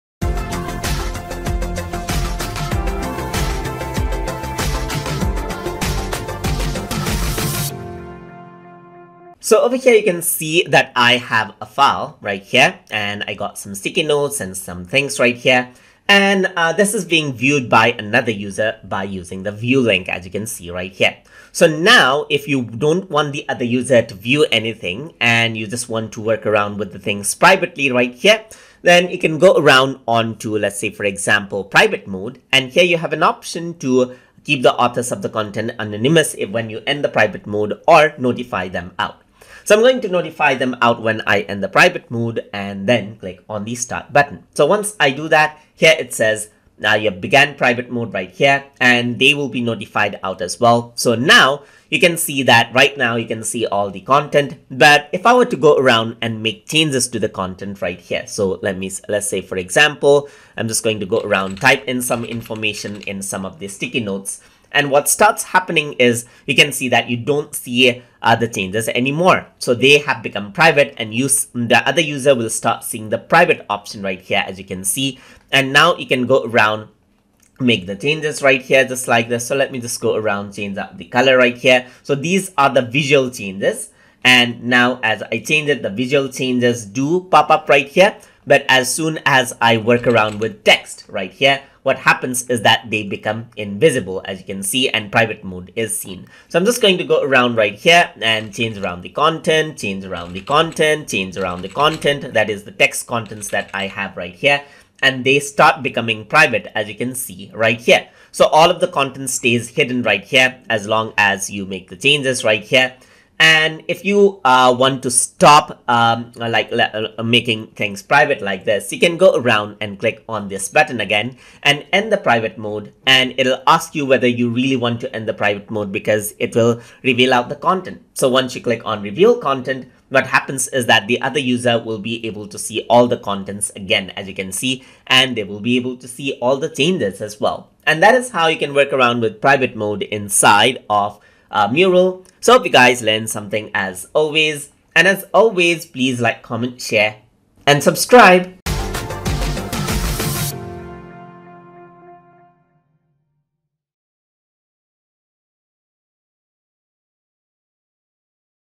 Mural Tutorial - Lesson 43 - Private Mode
In this tutorial, we will be discussing about Private Mode in Mural

Learn the basics of using Mural including how to work with color, shapes text, layout and collaborate with others.

This lesson has been made using Mural app in Google Chrome but is applicable for other supported web browsers as well.